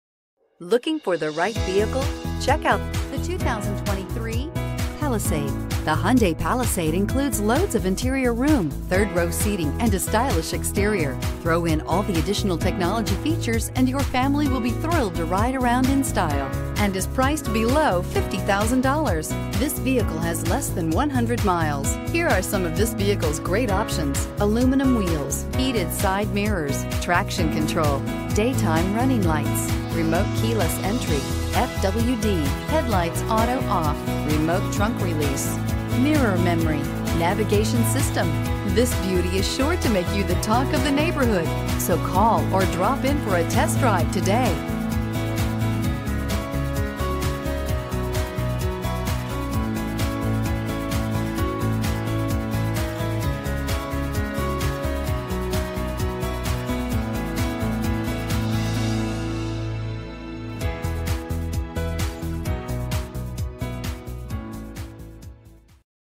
2023 Hyundai Palisade Spartanburg SC H23163
2023 Hyundai Palisade Limited

Benson Hyundai
2550 Reidville Road

Graphite 2023 Hyundai Palisade Limited FWD 8-Speed Automatic with SHIFTRONIC V6 19/27 City/Highway MPGbrbrRecent Arrival! 19/27 City/Highway MPG